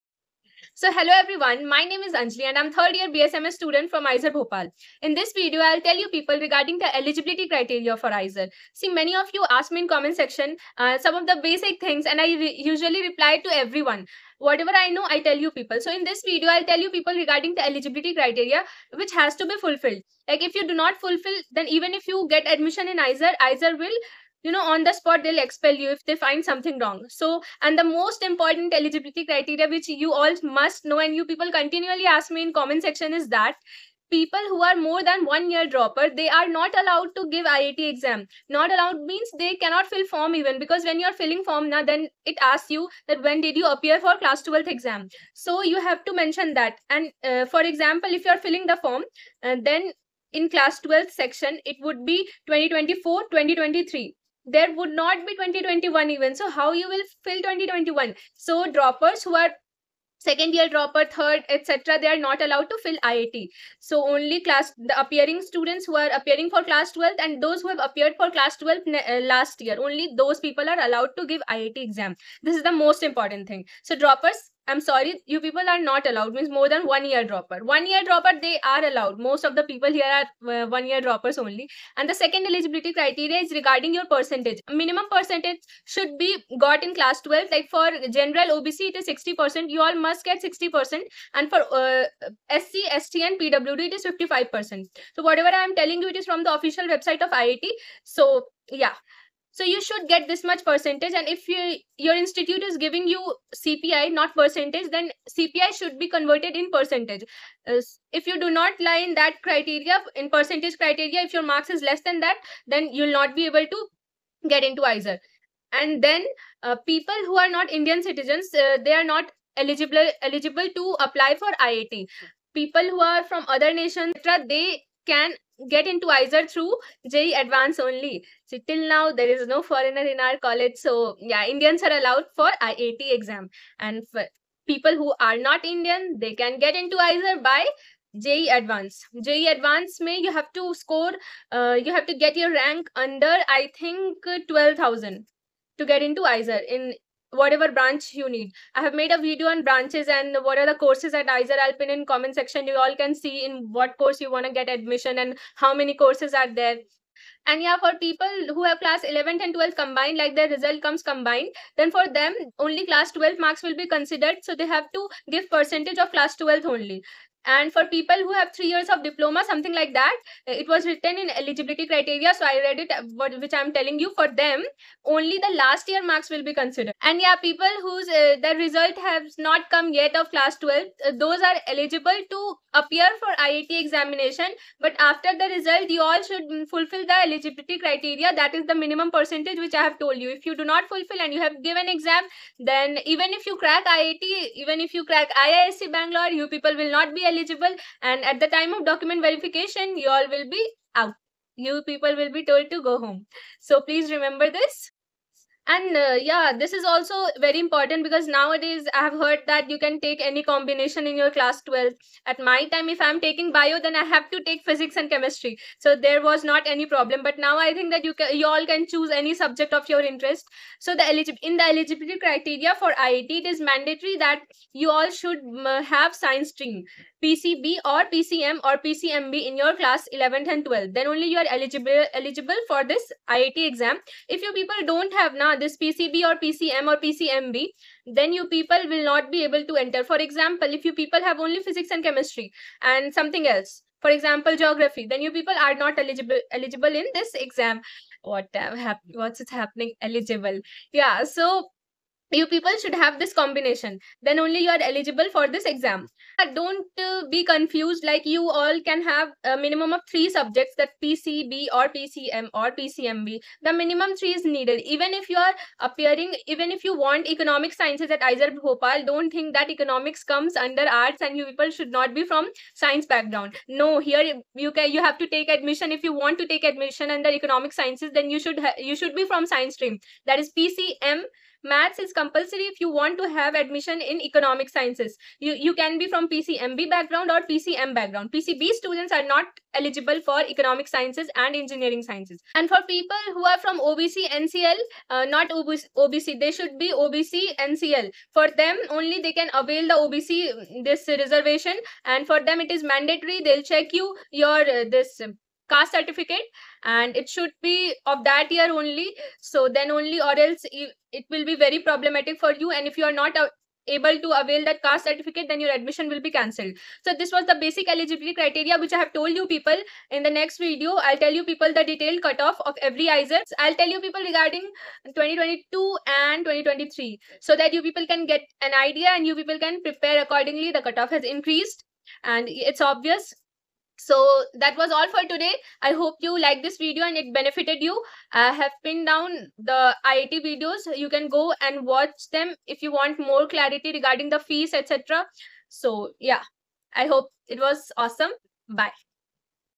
🔴Eligibility criteria for IAT(IISER)🔴iiser
Hello everyone I have told you all regarding the basic eligibility criteria.